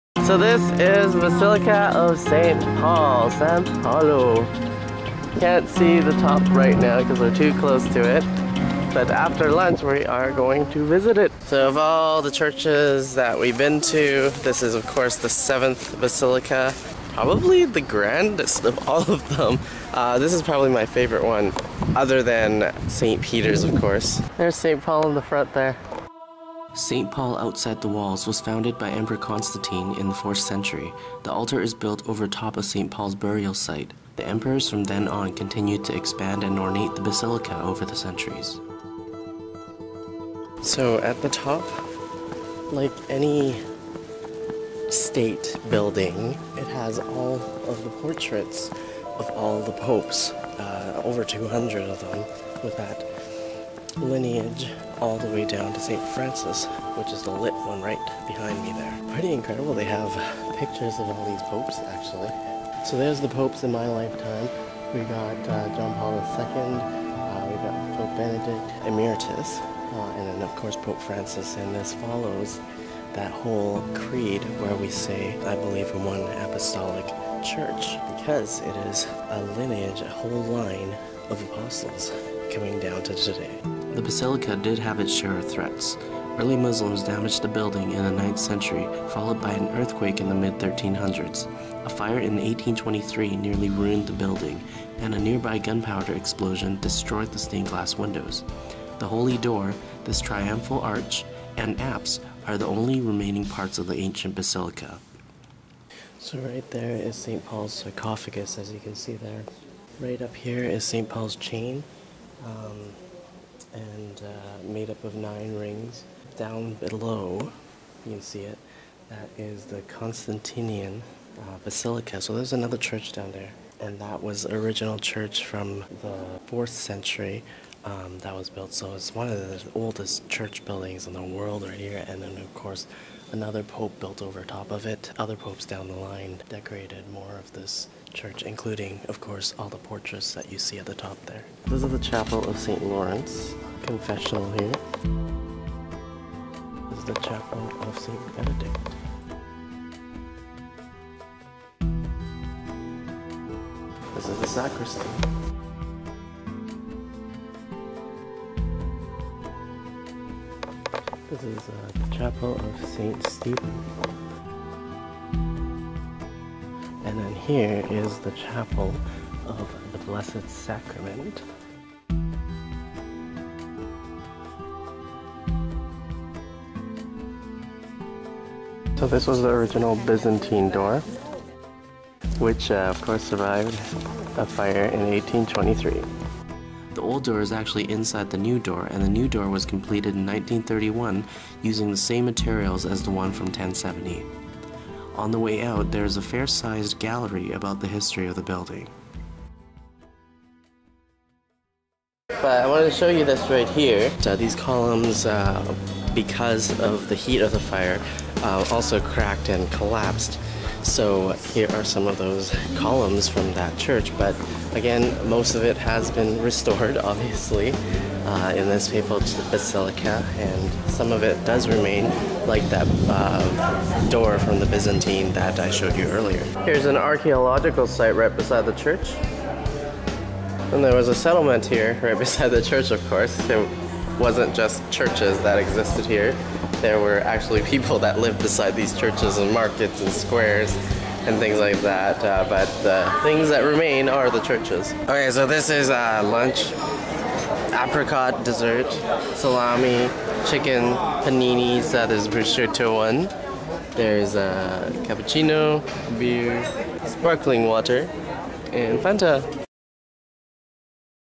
Papal Basilica of St Paul Outside the Walls (ROMEing Around 37)
Episode 37 of ROMEing Around, a Millennial Pilgrimage to Italy. In this episode, we visit the most beautiful of the Papal Basilicas, San Paolo fuori le Mura (Saint Paul Outside the Walls) with a brief history.